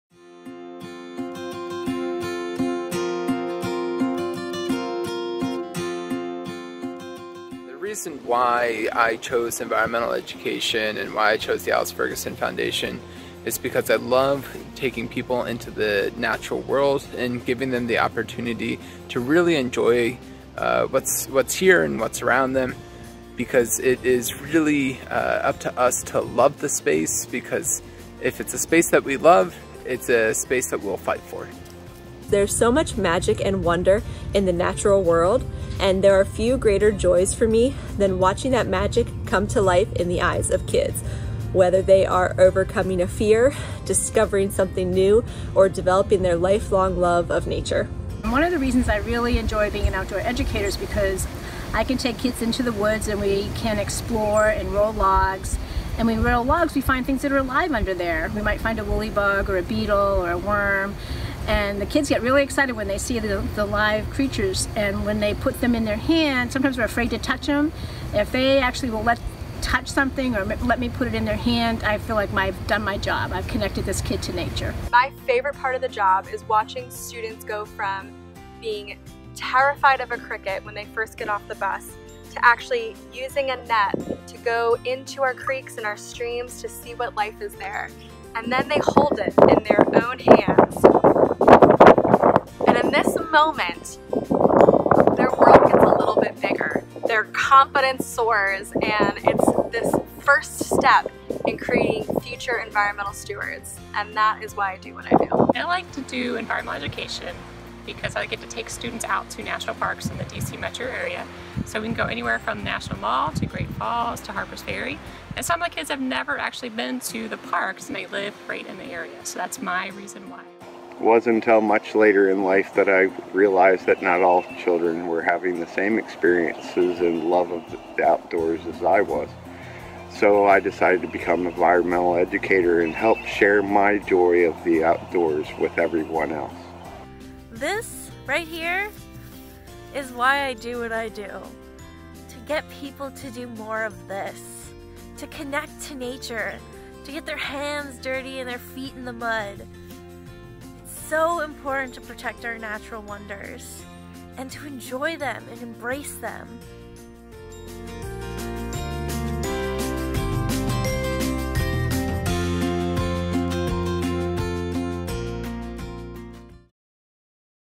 Why Environmental Education Matters
The educators of the Alice Ferguson Foundation share why they chose to be an environmental educator.